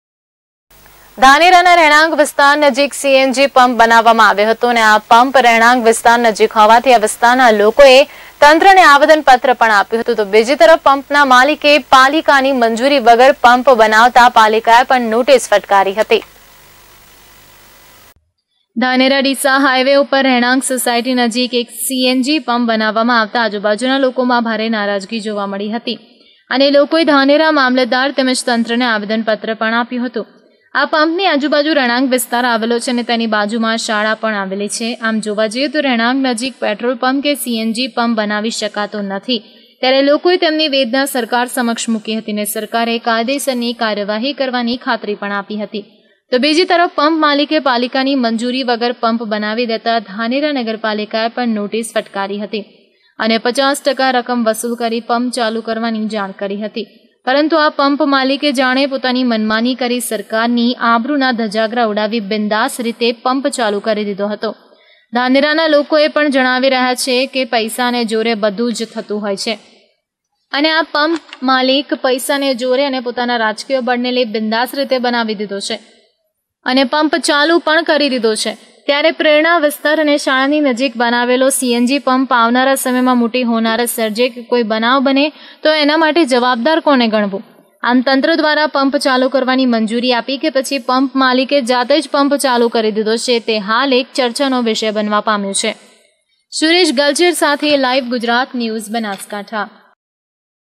બનાસકાંઠા :-પાલિકાની મંજૂરી વગર પંપ બનાવતા પાલિકાએ નોટિસ ફટકારી
ધાનેરાના રહેણાંક વિસ્તાર નજીક સીએનજી પમ્પ બનાવવામાં આવ્યો હતો. આપ પંપ રહેણાંક વિસ્તાર નજીક હોવાથી આ વિસ્તારના લોકોએ તંત્રને આવેદનપત્ર પણ આપ્યું હતું. તો બીજી તરફ પંપના માલિક એ પાલિકાની મંજૂરી વગર પંપ બનાવતા પાલિકાએ પણ નોટિસ ફટકારી હતી. પરંતુ લોકોનું આવેદનપત્ર અને પાલિકાની નોટિસ પાણીમાં ગઈ હોય તેવું લાગી રહ્યું છે. અને આ પંપ માલિકે બિન્દાસ થી સીએનજી પંપ ચાલુ કરી દીધો..જોઈએ શું છે સમગ્ર મામલો..